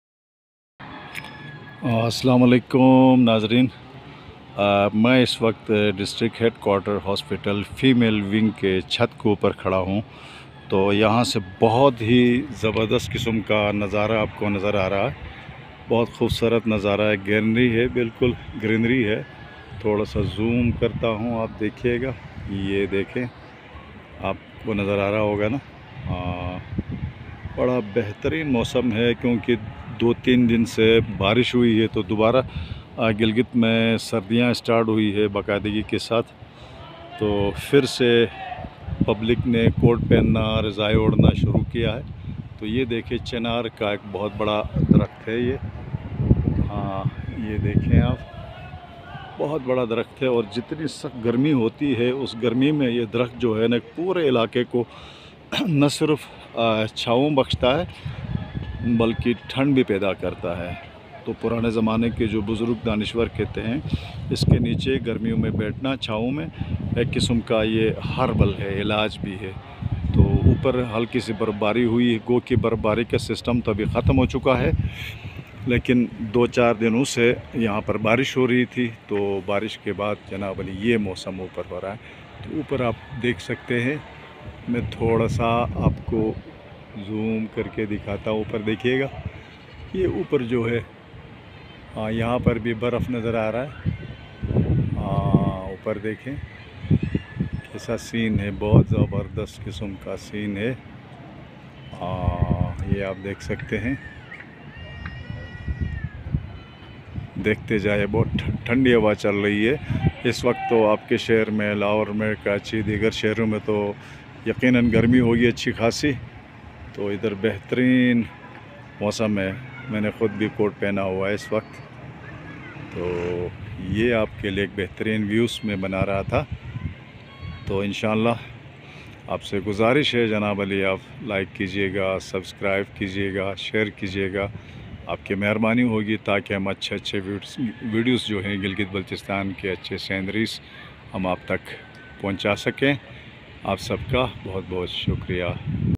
Beautiful view of Gilgit#
Beautiful view of Gilgit. The weather is very beautiful and after the rain the beauty of the area has increased. Gilgit is my heaven.